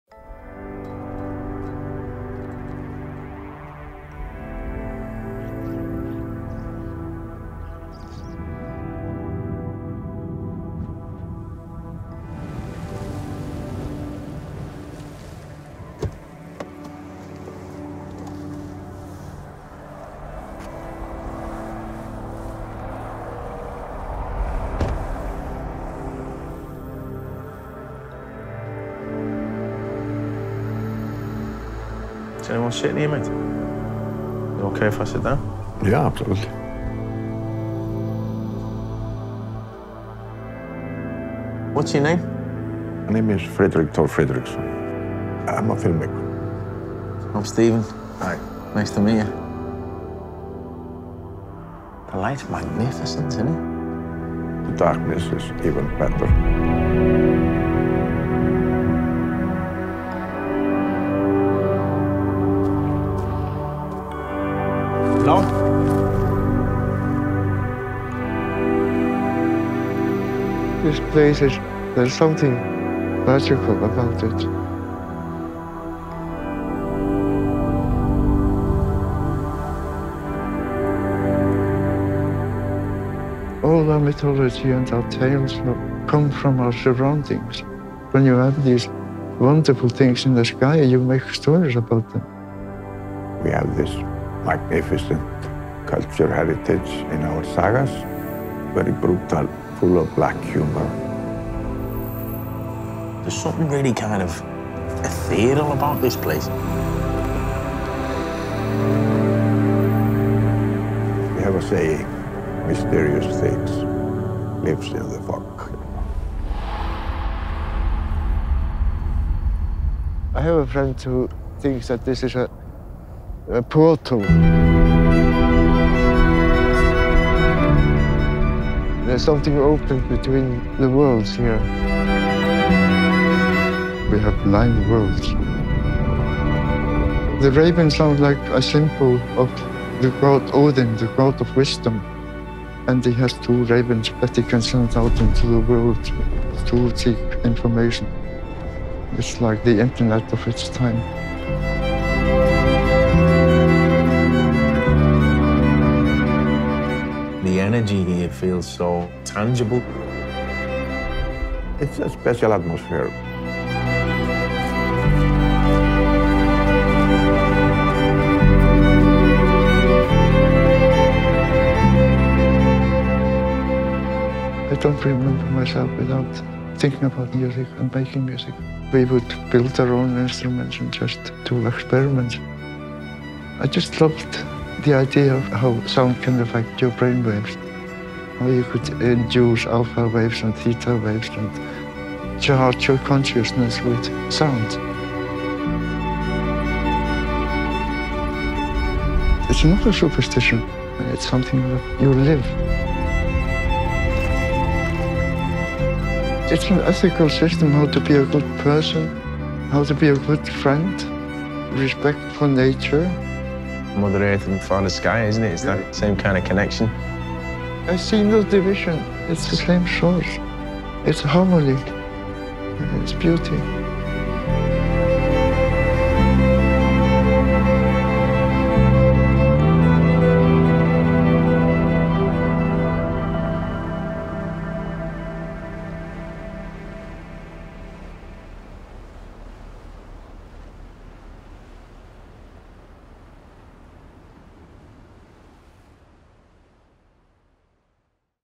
Pre-Spring 2023 SPEZIAL Collection | adidas Originals
In celebration of the remarkable connection between humans and nature, adidas Spezial and Stephen Graham embark on an unexpected journey in the wilds of Iceland.​

The latest adidas Spezial collection is available now through CONFIRMED and select retailers.